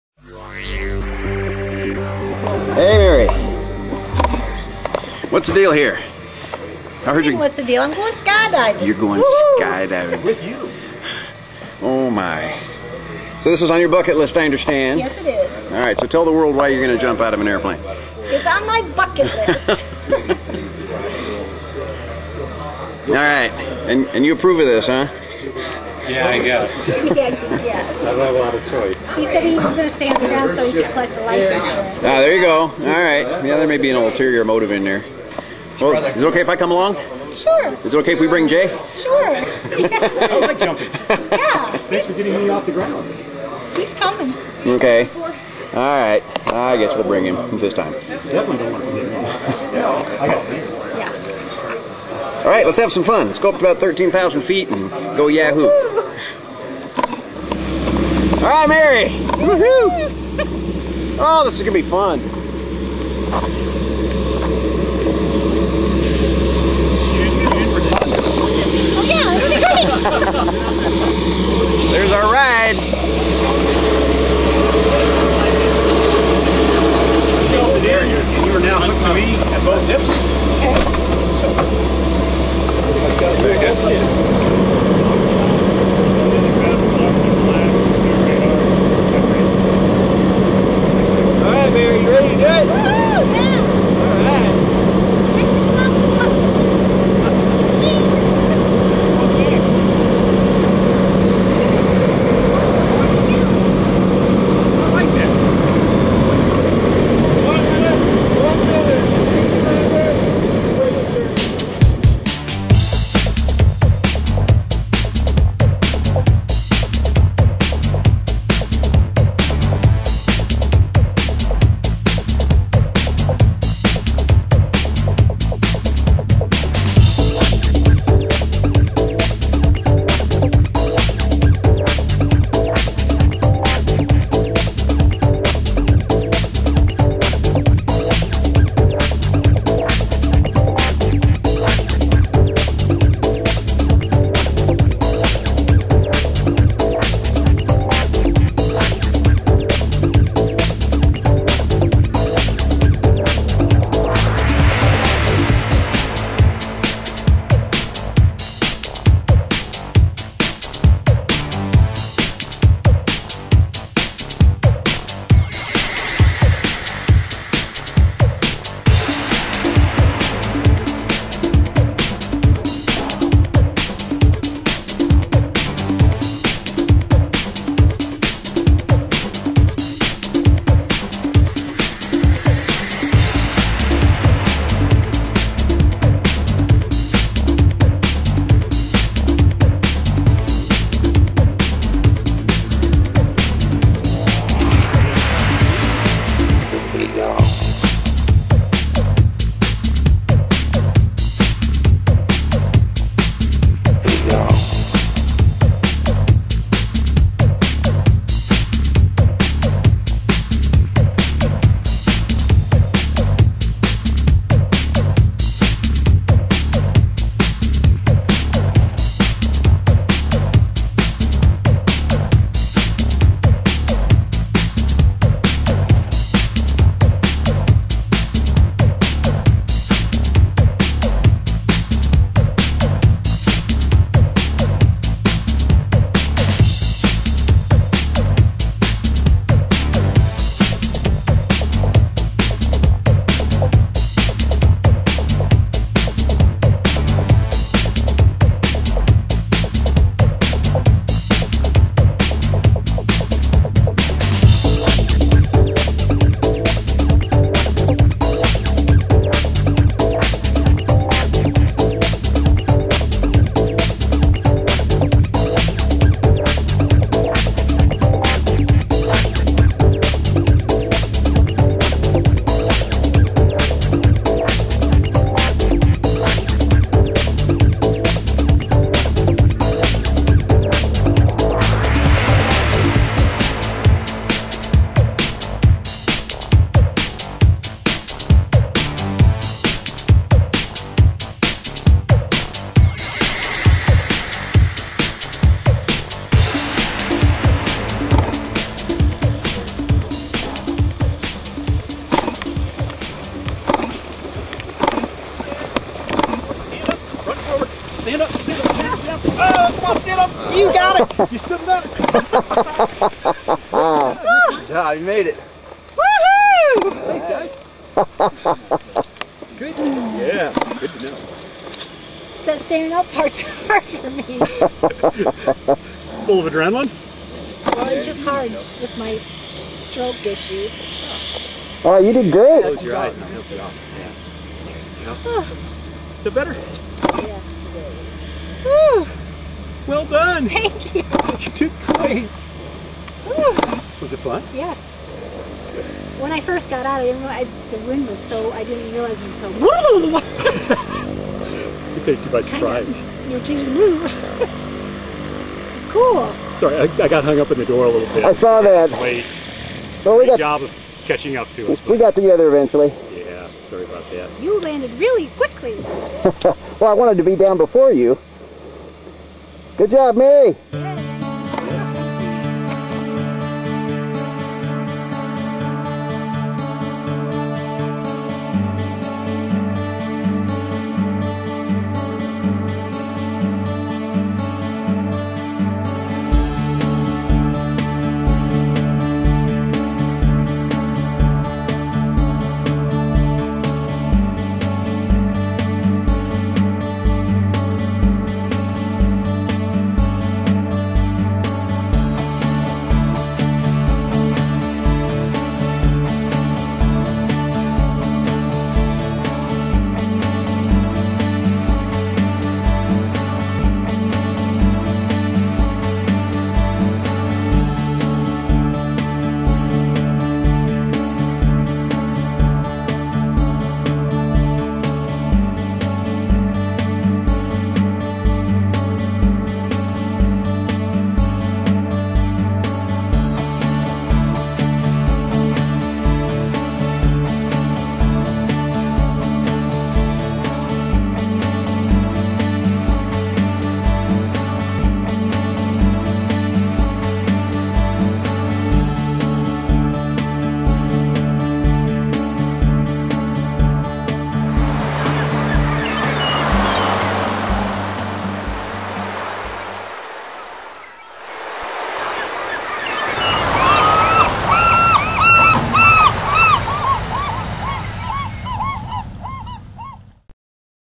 Mary Crowley goes skydiving!
Mary Crowley makes a tandem skydive at Paradise Skydives in Vinton Iowa on saturday, June 24th, 2010 with instructor Jay Speckeen